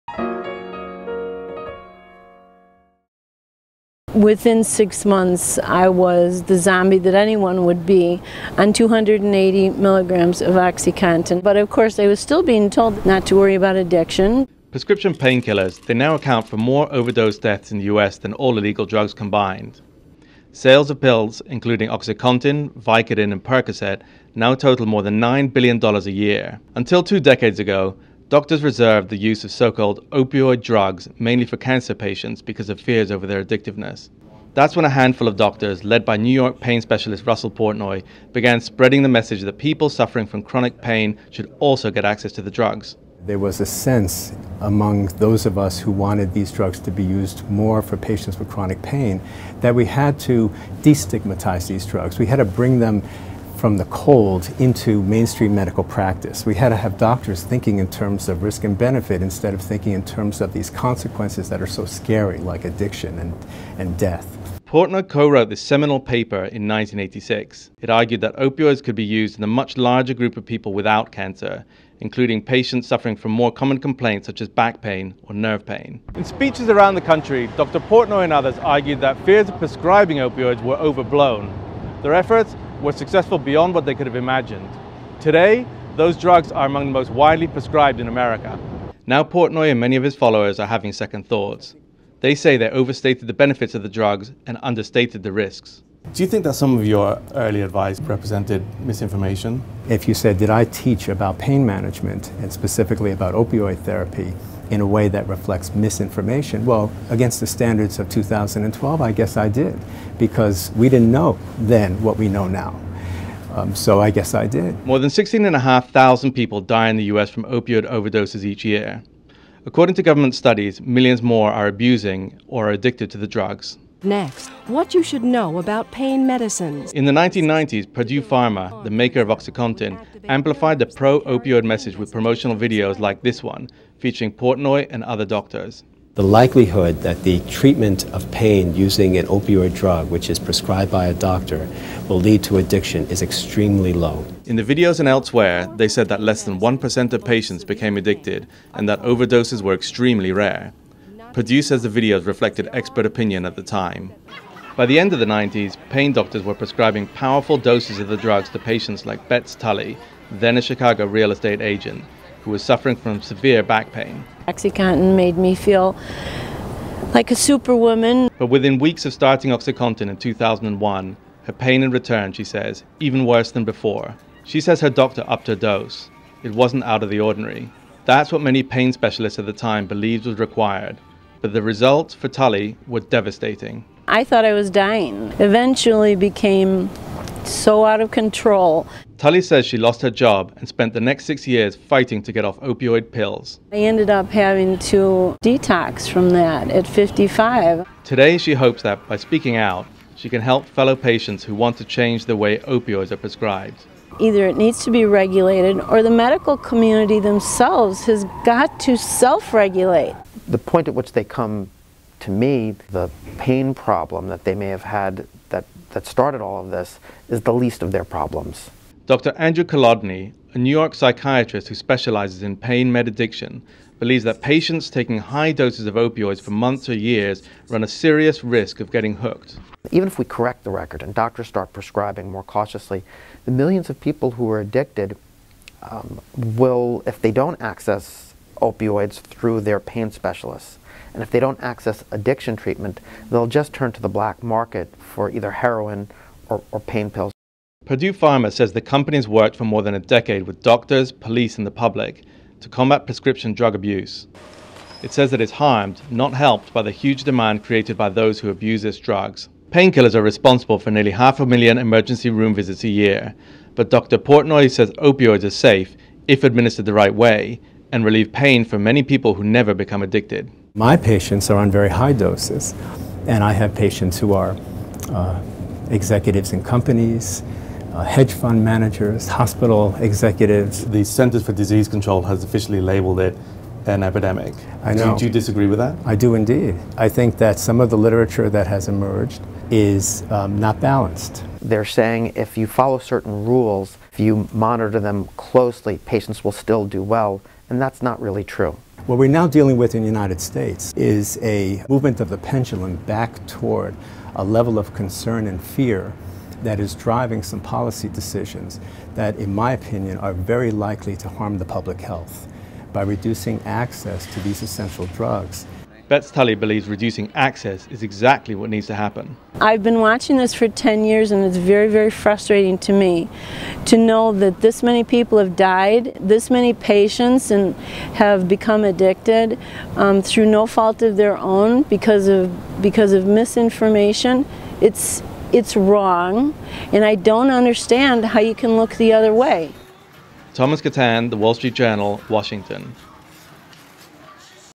Thousands Die Annually From Pain Med Overdose
More than 16,000 people die from opioid overdoses every year. Now, Dr. Russell Portenoy, who campaigned for wider prescription of pain medications like Vicodin, Oxycontin and Percocet, is having second thoughts. WSJ's Thomas Catan reports. Photo: Bryan Thomas.